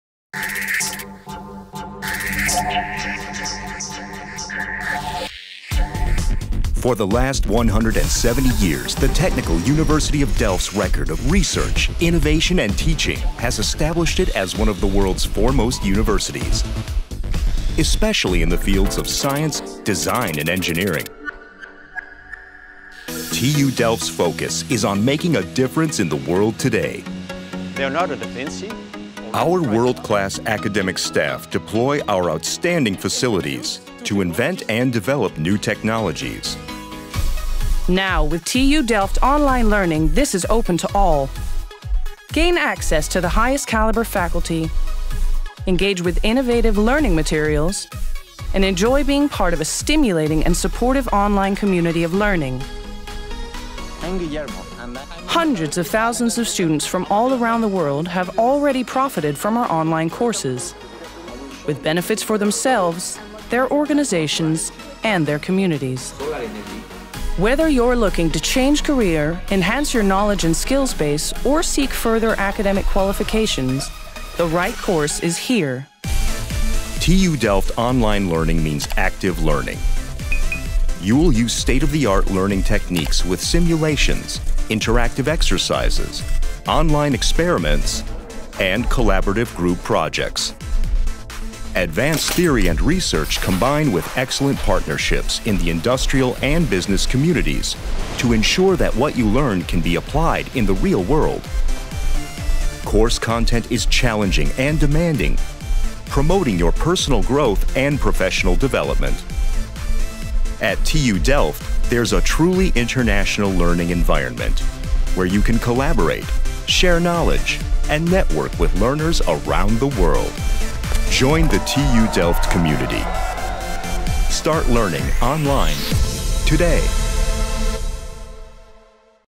TU Delft Online Learning
Are you looking to make your career in science, design or engineering? Then join the community of online learners at TU Delft!
At TU Delft, online learning means active learning. Our courses are designed to provide you with an engaging learning experience. Course content is challenging and demanding, promoting your personal growth and professional development, while enjoying the flexibility and accessibility that our online courses offers so you can combine learning with other priorities of your life.